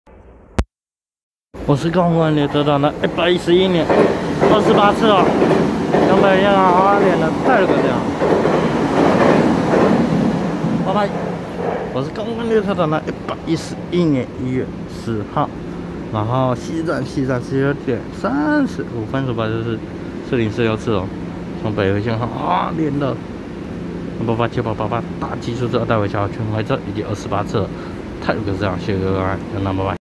4046次湖口發車經由北迴線開往花蓮的EMU888+EMU887大吉數字二代微笑號區間車，和248次樹林發車經由北迴線開往花蓮的太魯閣自強號
111/1/10汐止站19:32分拍攝4046次湖口發車經由北迴線開往花蓮的EMU888+EMU887大吉數字二代微笑號區間車，和248次樹林發車經由北迴線開往花蓮的太魯閣自強號